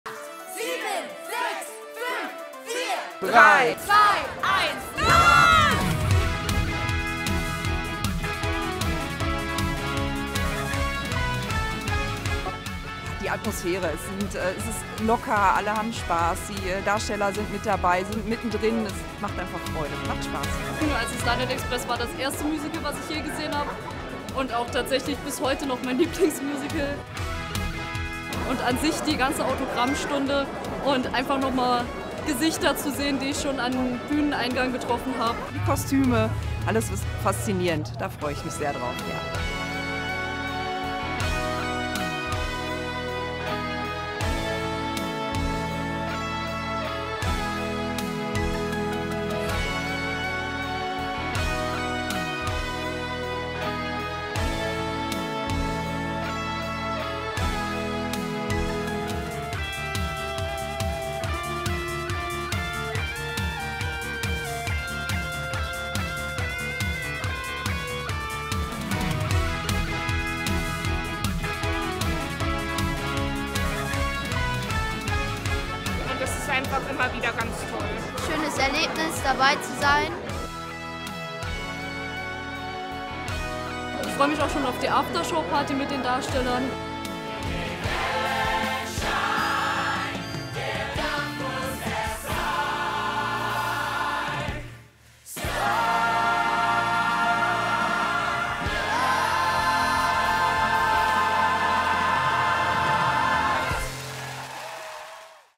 STARLIGHT EXPRESS - Rennen in den Mai 2023
Am 30. April 2023 ging es wieder rasant zu beim Rennen in den Mai. Viele tolle Attraktionen, wie Meet & Greet, Licht- und Technikshow, eine spektakuläre Show und eine After Show-Party haben die Besucher*innen fasziniert.